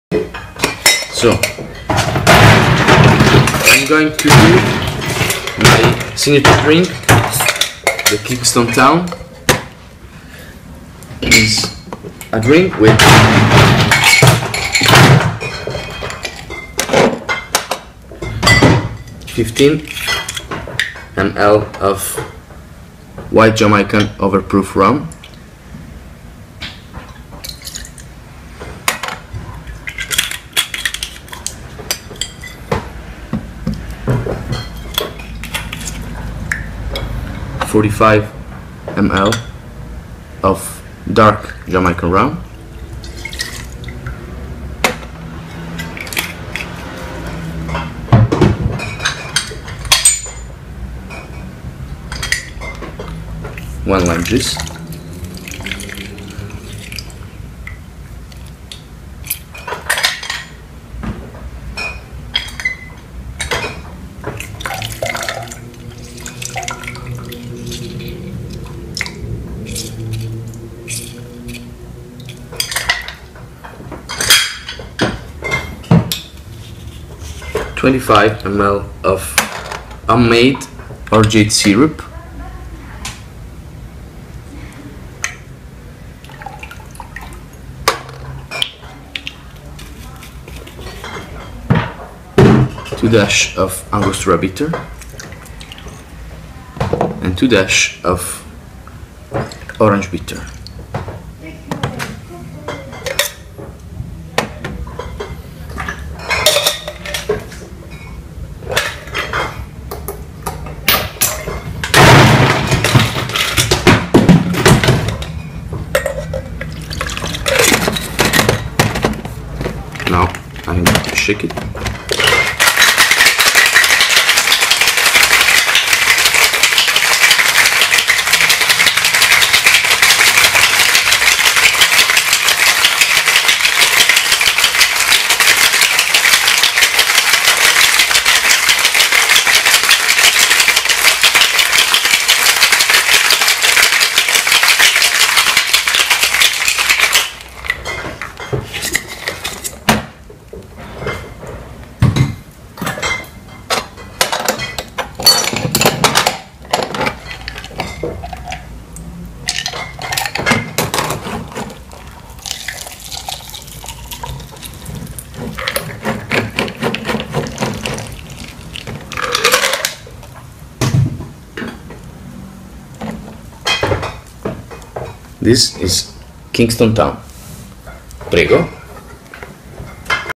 Giuseppe Tatone from Luau Tiki Bar (Bari) : Kingston Town
Giuseppe Tatone from Luau Tiki Bar makes for us his signature cocktail the Kingston Town